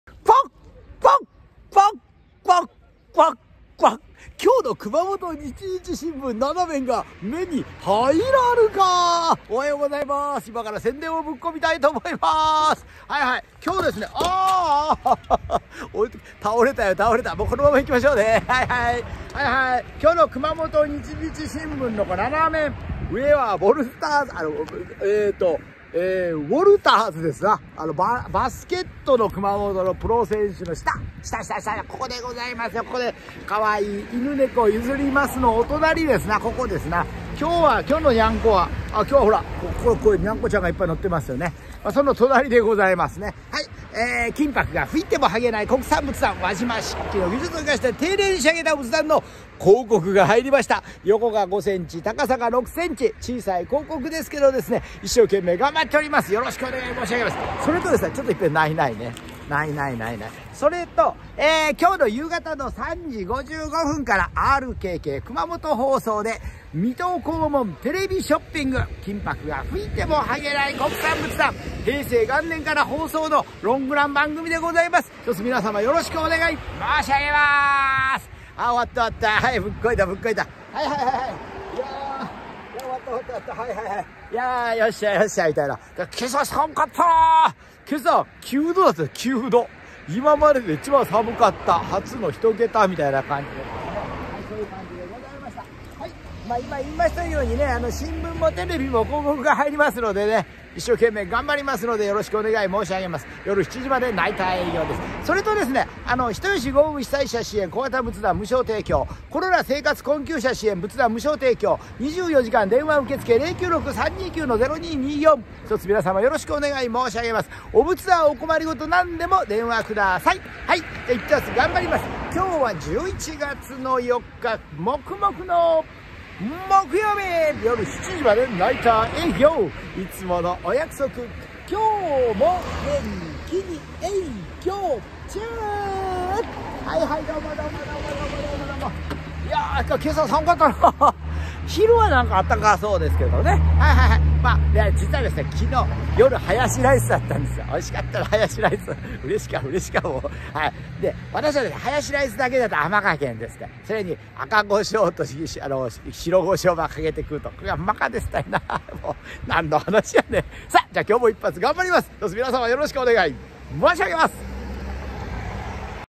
熊本 仏壇店 水戸黄門コスプレ社長 RKK水戸黄門テレビCM 熊本日日新聞広告 金箔が拭いてもハゲない国産仏壇
(有)輪島漆器仏壇店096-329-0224熊本市西区上高橋1-11-2年中無休24時間電話受付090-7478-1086#Shorts